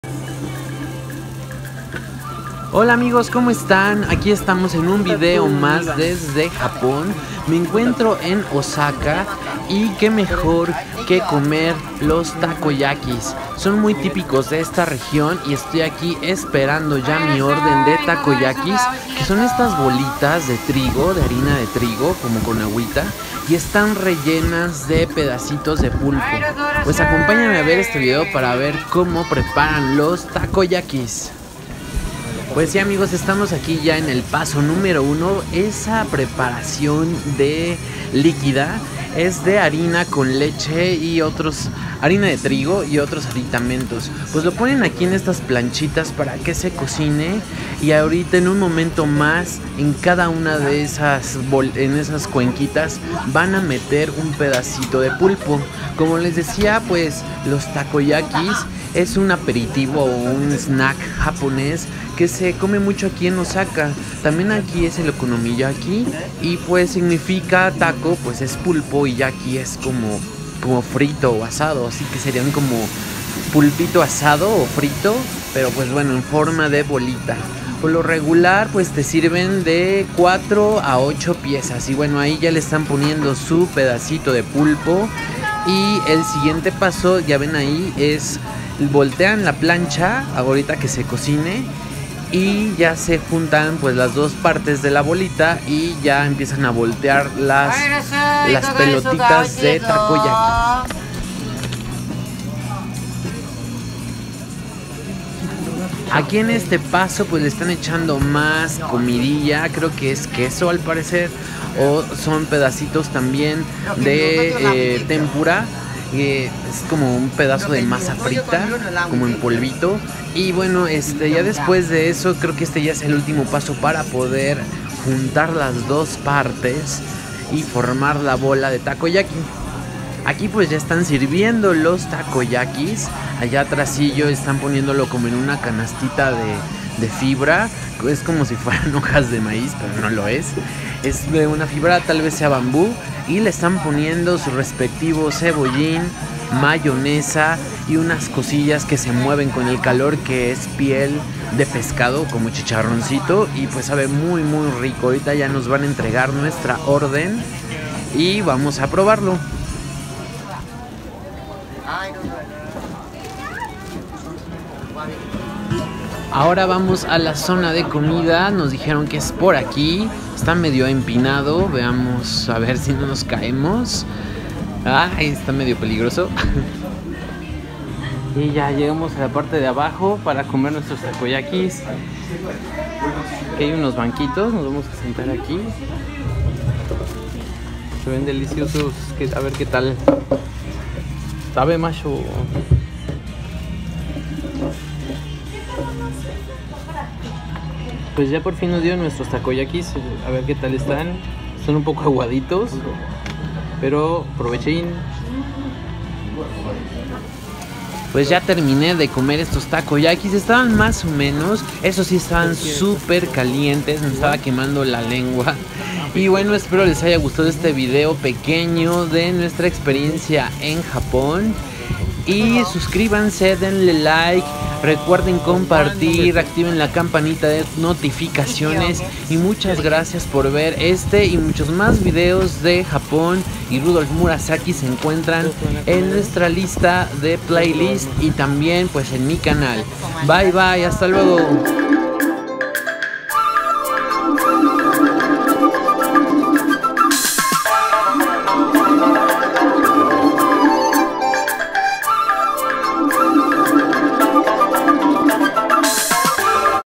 TAKOYAKI STREET FOOD en OSAKA / Comiendo BOLITAS DE PULPO たこ焼き
Fui a un puesto callejero en OSAKA y comi estos ricos Takoyakis o bolitas de fritas de pulpo muy cerca del rio y de Dotonbori